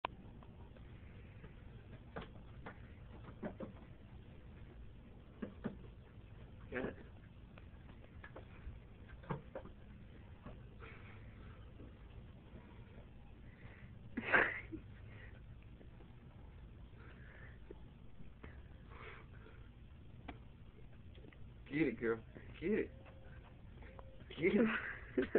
8 week old pit playing ! :)
ny brother playing with Nina !